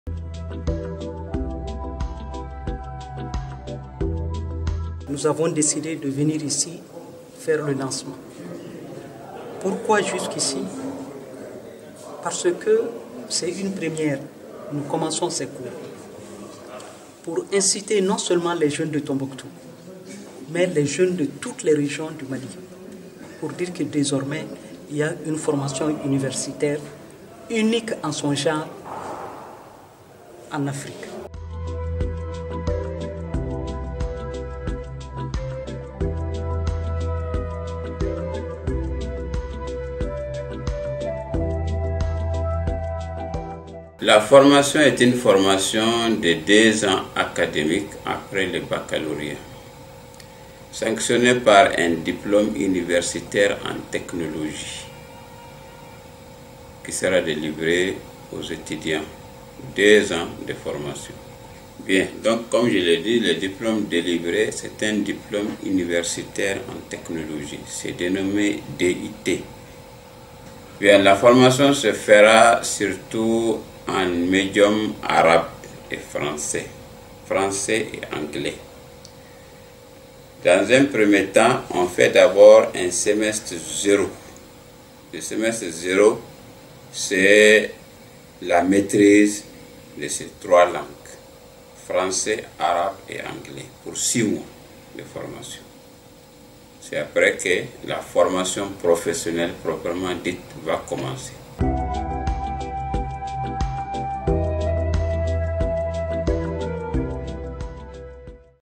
Manuscrits anciens : une formation universitaire lancée à Tombouctou
Les activités académiques de l’Institut des hautes études et de recherches islamiques Ahmed Baba ont été lancées le 23 octobre dernier à Tombouctou. Selon Abdoulaye Cissé, secrétaire général de l’Institut, il y a trois filières de formation dans les domaines suivants: la codicologie, la numérisation et la conservation physique des manuscrits. Une vingtaine d’étudiants se inscrits et attendent le démarrage des cours.